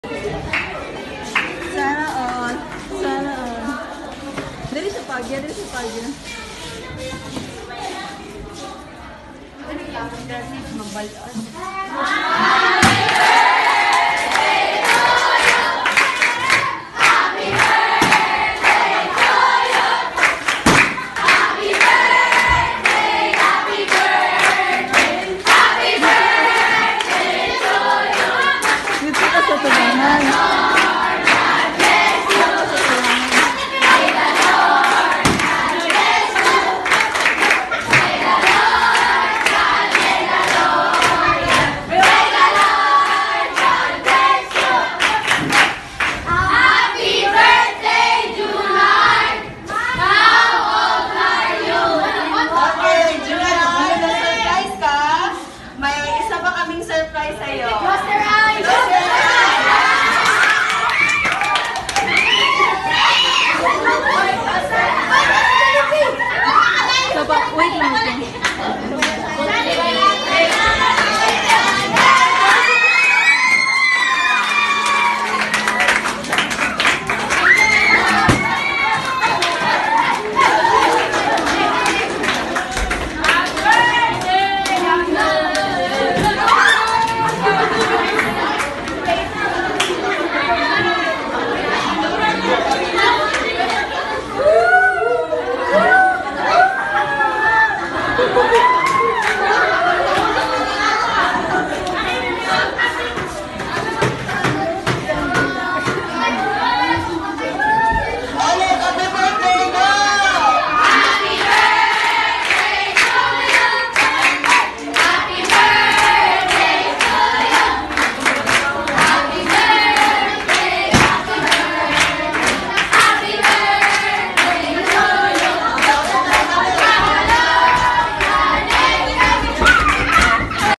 Pinay OFW sa singapore umuwi ng pinas para surpresahin ang anak sa kanyang special na kaarawan.
A very touching moments